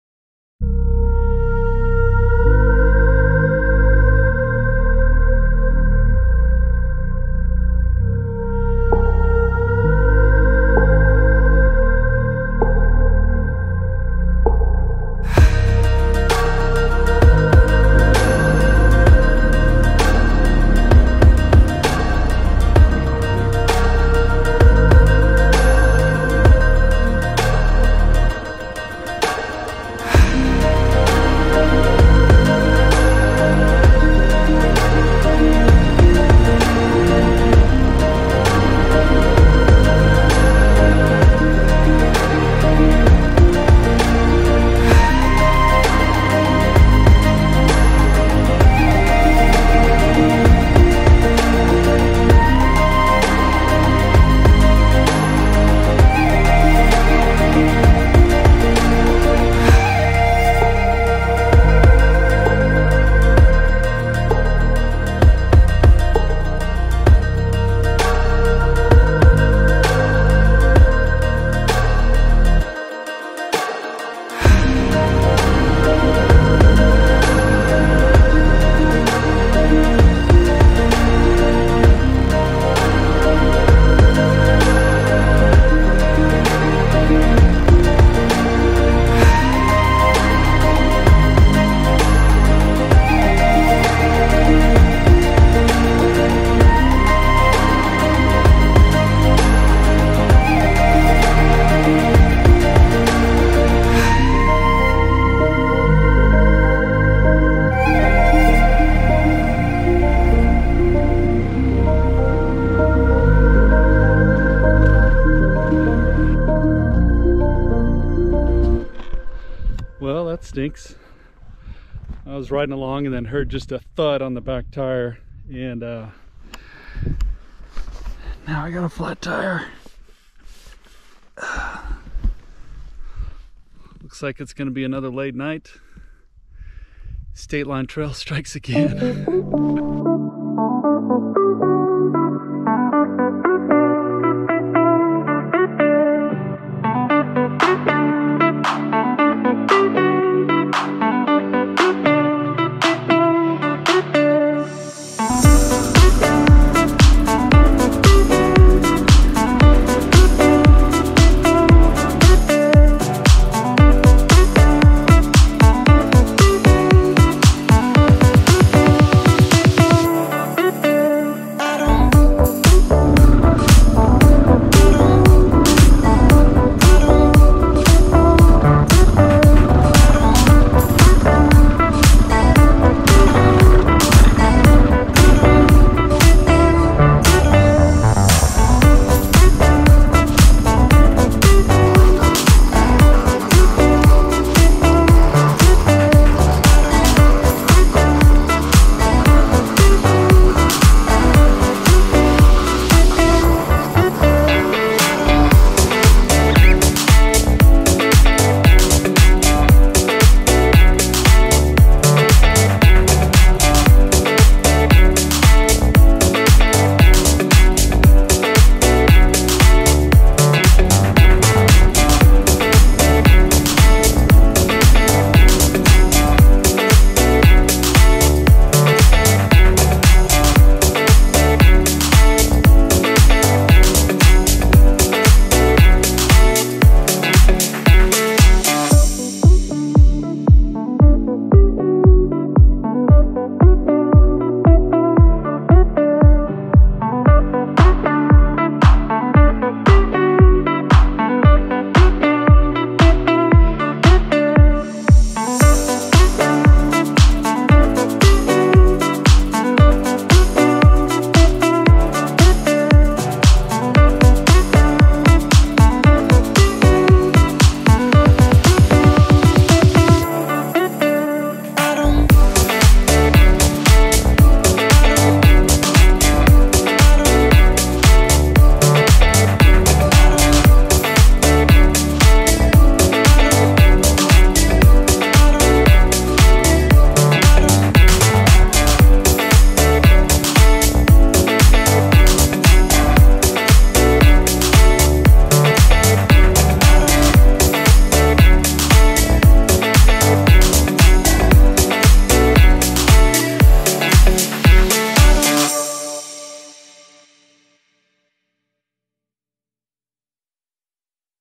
Stateline Trail Ride HD 1080p
150 mile ride through Montana/Idaho mountains.